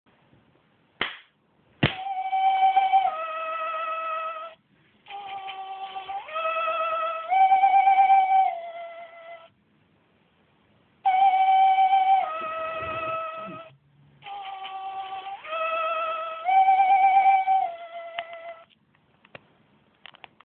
Real Life Gizmo
A Real Gizmo that was found in chinatown London in an alley under some newspaper.